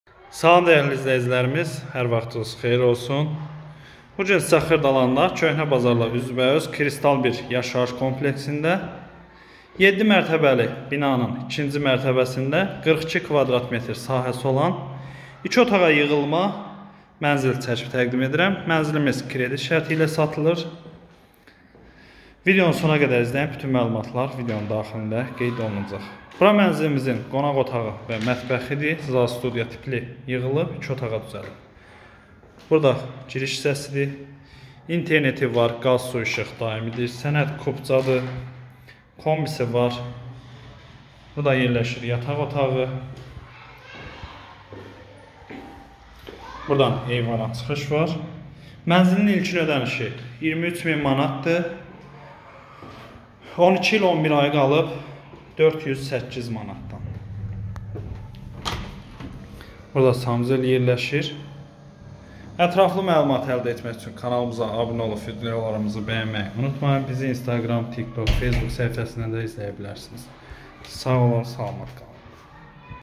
KreditEv 7mertebede 2 otaqa düzelme (055)339-68-99 Elşən Xirdalan Kristalda 7/2 42 kv
Xırdalanda kirisdal 1 yaşayış kompleksində 7/2 ci mərtəbəsində 42 kv m sahəsi olan 2 otağa düzəlmə mənzil kireditlə satılır. Sənət kupca. İlkin 23000 manat 12 il 11 ay 408 manatdan